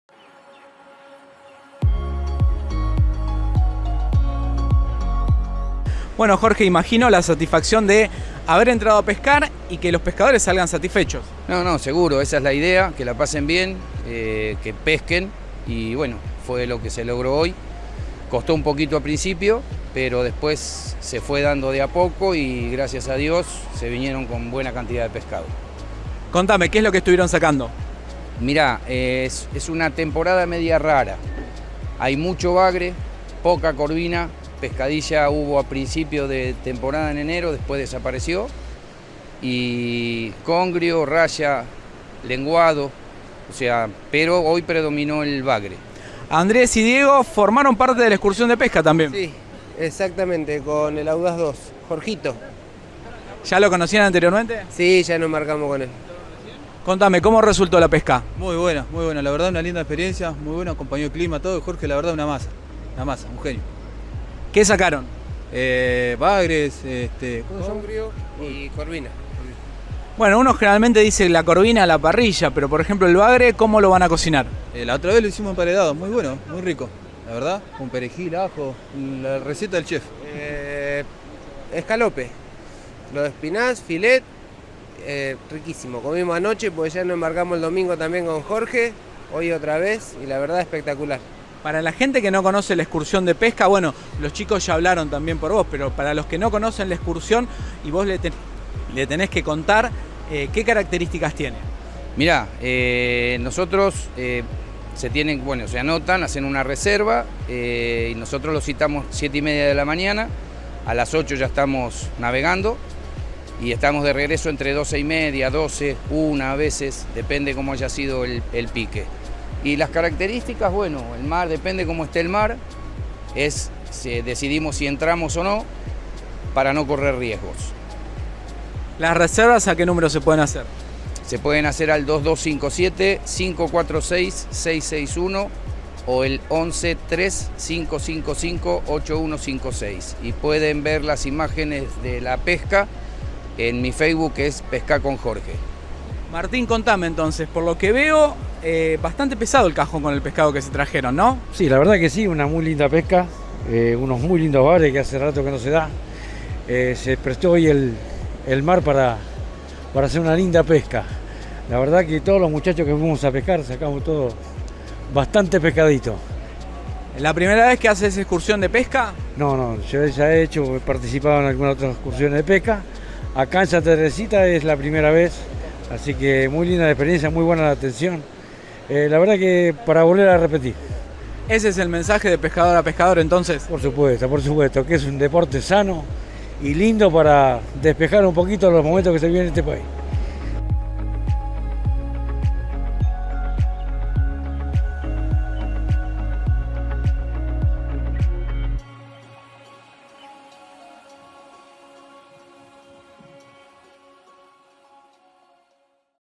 Viví la experiencia de la pesca embarcada
El Audaz ofrece excusiones todos los días desde la bajada de Playa y calle 50 en Santa Teresita.
La actividad incluye equipo, carnada y asesoramiento. También ofrece servicio de limpieza y freezer.
Podés reservar tu lugar a través del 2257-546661 o el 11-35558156.